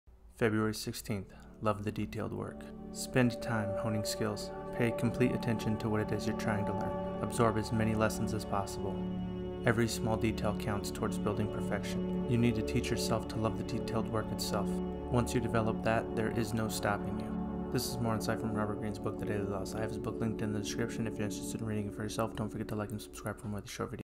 February 16th: Love the Detailed Work
This is my take on the book ( The Daily Laws by Robert Greene)
48 laws of power
I read his book when it came out back in October.
I felt like more people need to know what was inside. I'm only giving my short thoughts on what's in the pages, I would highly suggest checking it out yourself.

I use this from time to time when my eyes get tired or I need a little more light while reading.

Video by David Roberts from Pexels